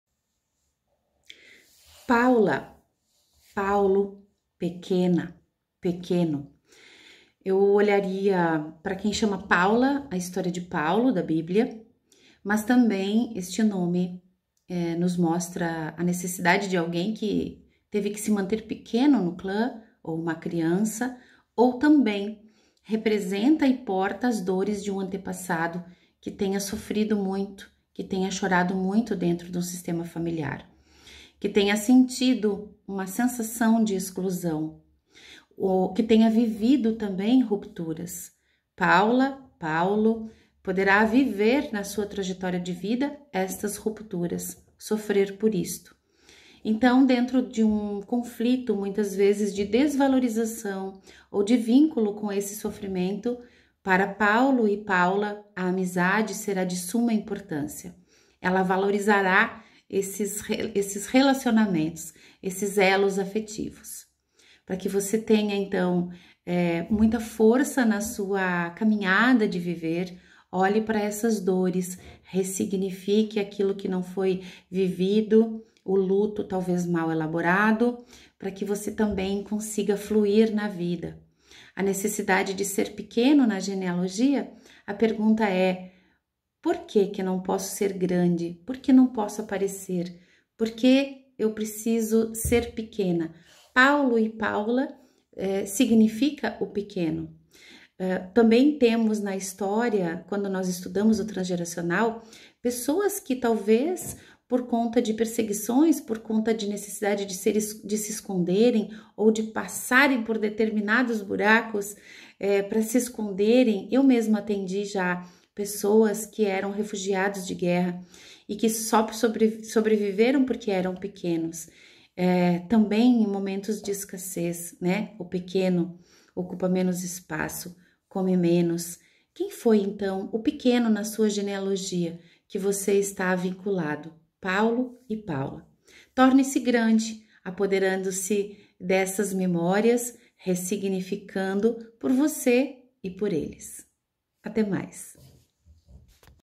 Significado nome PAULA/PAULO - Mandatos
Paulo(a), o(a) pequeno(a).
Paula(o) esconde o pranto de um membro do clã.
Olhar para a história de Paulo/Saulo, o apóstolo.
Que PATERNIDADE você veio reparar?

O que o dia 29 de JUNHO fala para você?